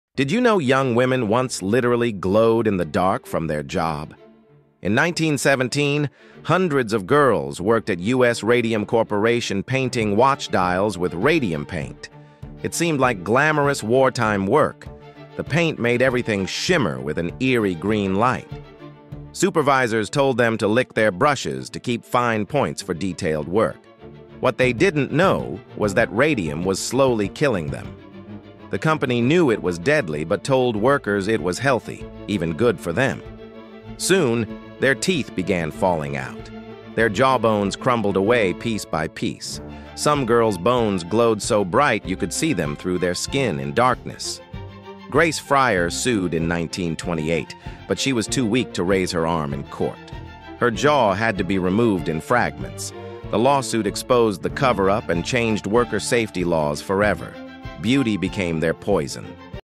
Women Who Literally GLOWED in the Dark from Their Jobs
radium girls who glowed through their skin... wild history